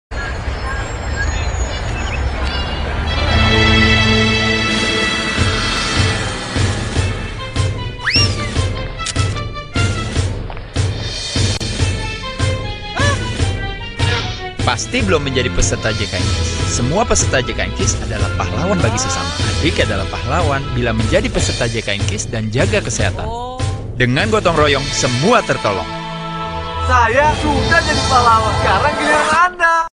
Iklan BPJS Kesehatan - Jadi Pahawan ft. Ade Rai (2017) @ iNews TV, Trans 7, TVRI
Iklan ini menghadirkan Ade Rai.

Ditayangkan di iNews TV, Trans 7, dan TVRI pada tahun 2017.